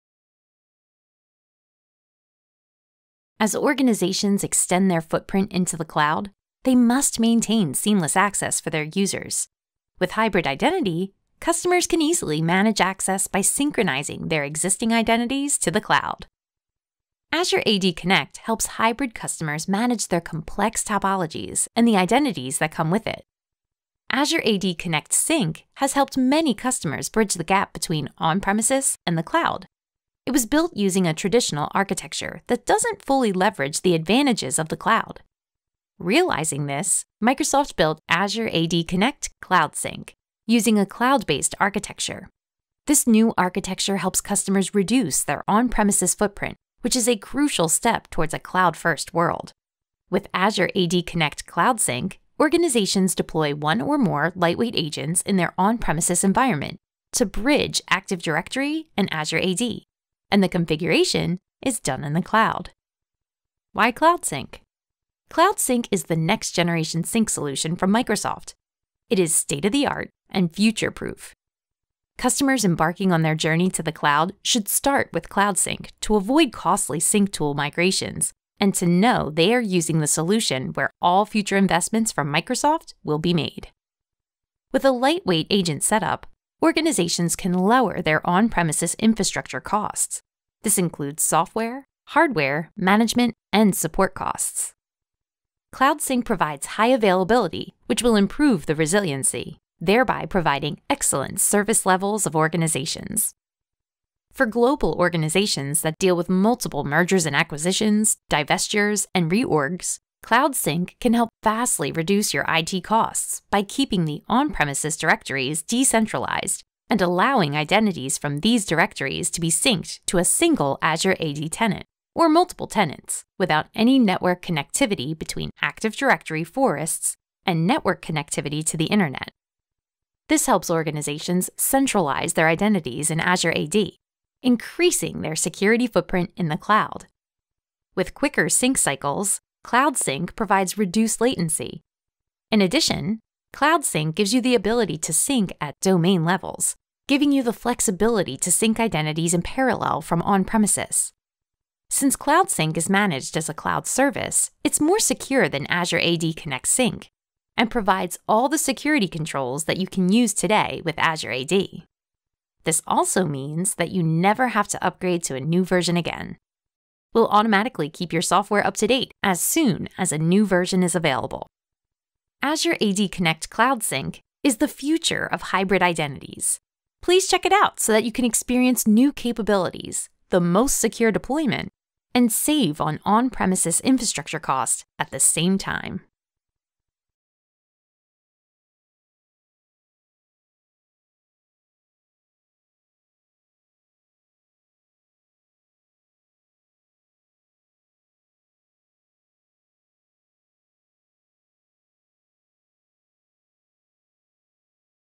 Entra ID Connect Cloud Sync overview
Microsoft Entra ID is the new name for Azure Active Directory (Azure AD).

Learn how to maintain seamless access as organizations move more into the cloud. Learn how to maintain seamless access as organizations move more into the cloud. With Hybrid Identity customers can easily manage access by synchronizing their existing identities to the cloud.